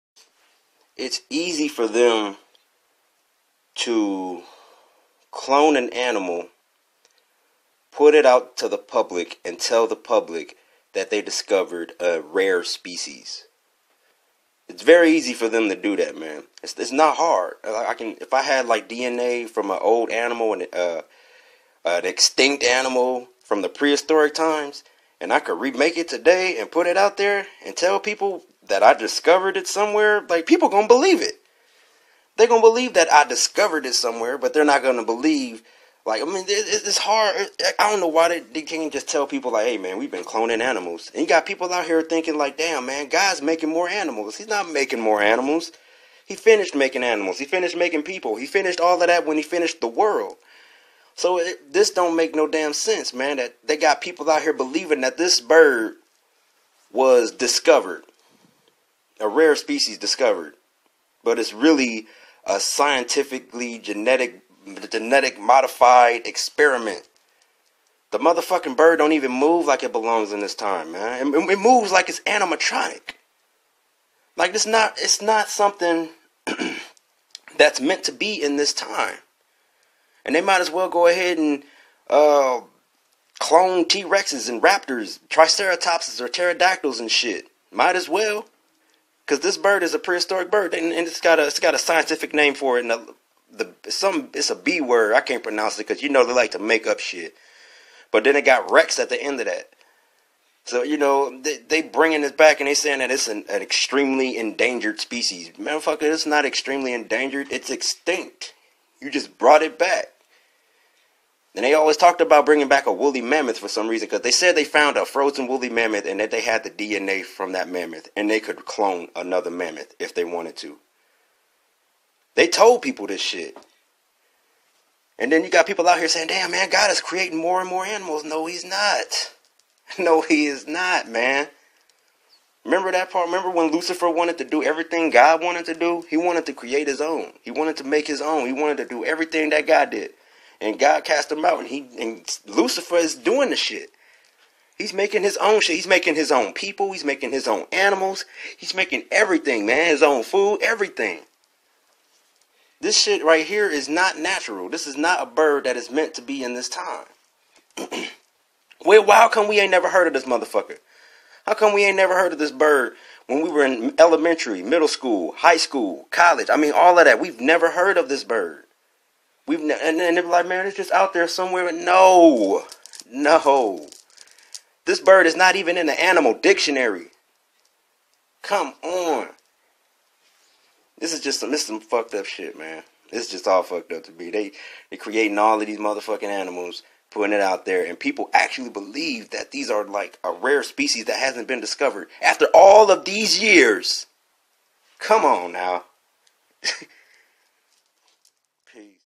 Resurrected Shoebill Stork
The cloned prehistoric shoebill. Yup , just like Jurassic Park.